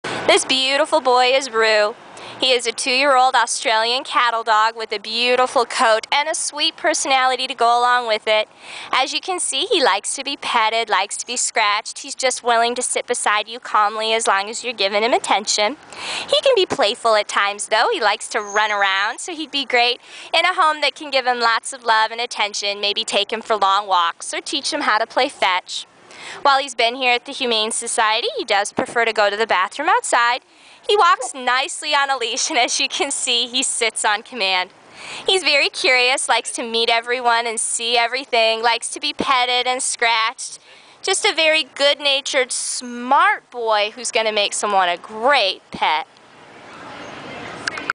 Rue-Available at the Humane Society of Missouri
RUE - ID#A485843

I am a neutered male, black and white Australian Cattle Dog.
The shelter staff think I am about 2 years and 1 month old.
I have been at the shelter since Apr 26, 2010.